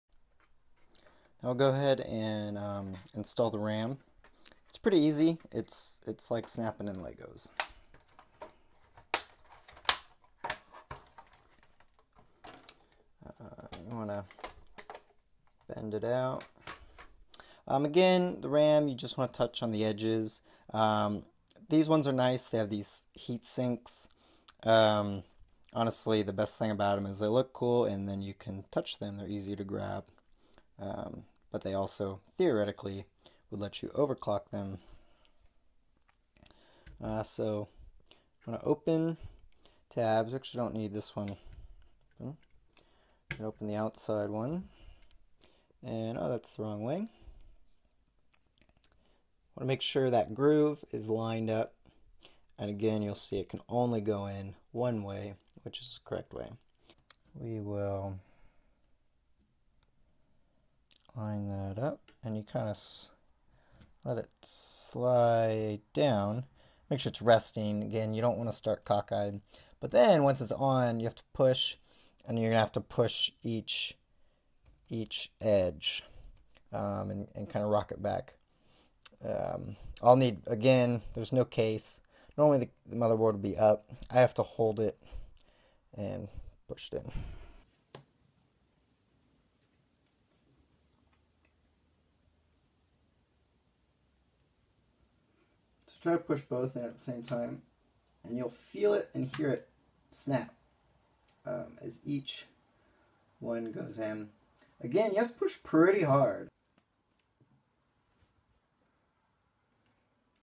How to install RAM
Installing a DDR3 stick of RAM into a H97 motherboard